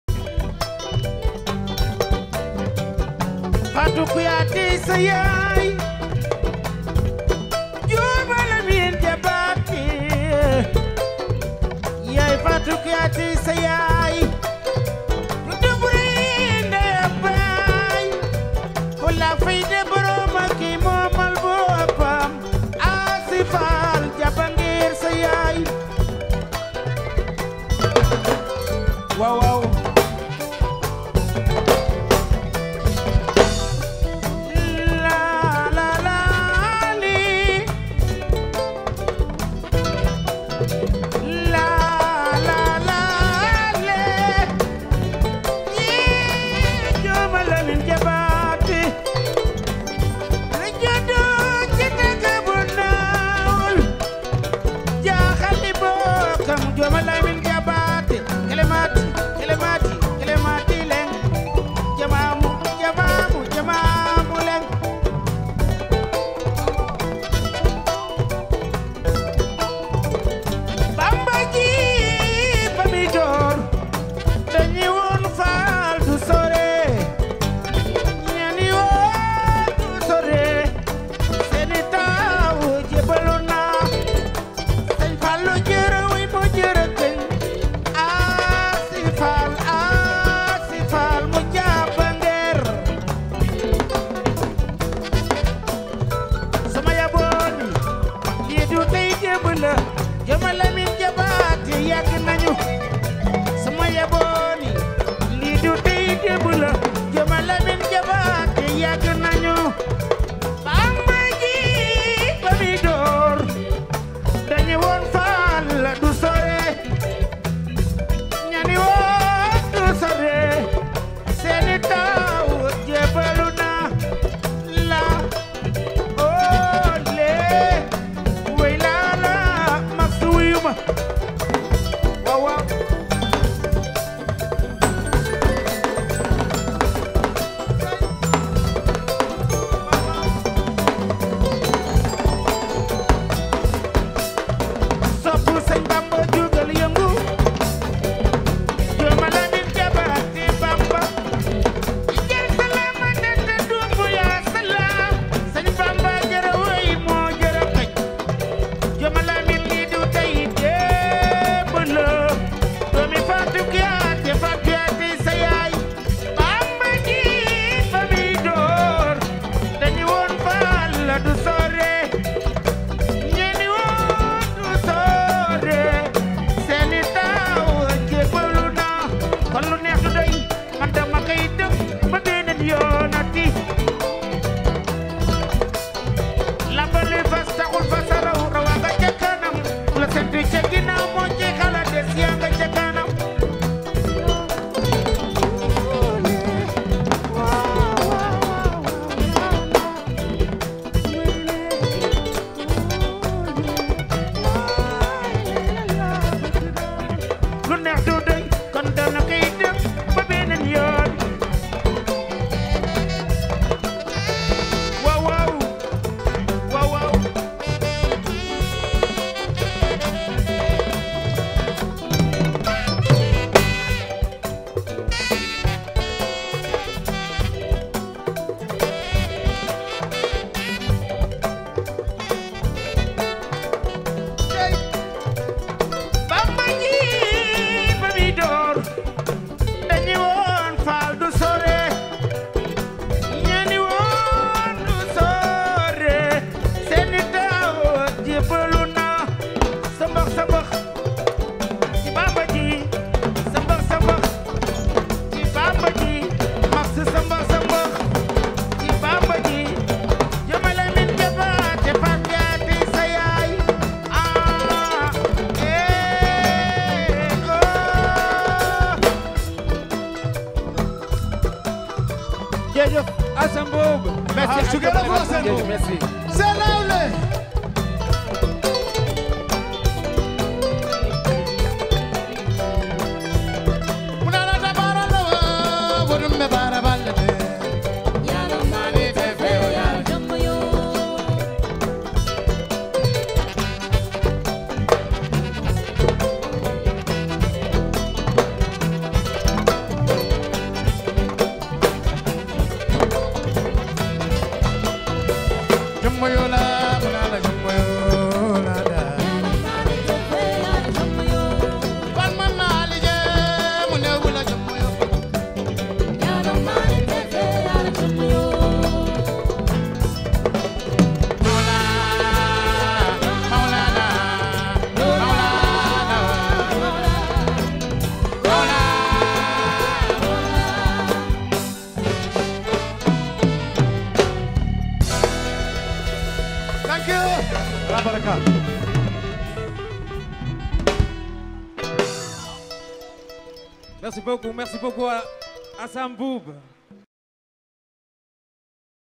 King Kora feat. Assane Mboup
Live at the CCF in Dakar. This concert was part of King Kora's Westafrica tour in April/May 2010 and the guest performance of Assane Mboup, also known as one of the singers of the Orchestra Baobab, was definitly one of the musical highlights of that tour.